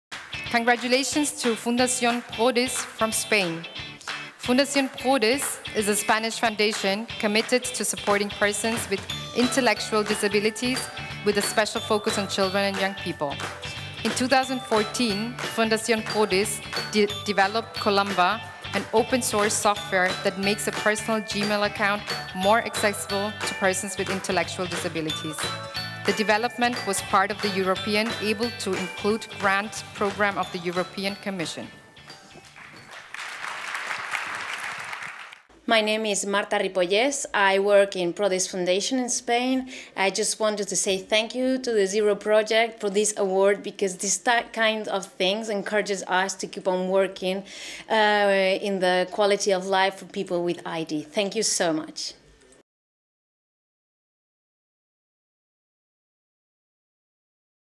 Fundacion Prodis: Making e-mail communication accessible for persons with intellectual disabilities
Welcome to the awardee video of Fundacion Prodis´ Kolumba software, a winner of the 2018 Zero Project Award on Accessibility.

Fundacion Prodis is a Spanish foundation committed to supporting persons with intellectual disabilities in the areas of training, education, culture, sports, and leisure, with special focus on children and young people. In 2014, Fundacion Prodis developed Kolumba – an open source software that makes a personal Gmail account more accessible to persons with intellectual disabilities. The development was part of the European ABLE TO INCLUDE grant programme of the European Commission, aimed at the introduction of communicational accessibility for people with intellectual disabilities in any software development.

Speaker:
Marta Ripollés, Prodis Foundacion, Spain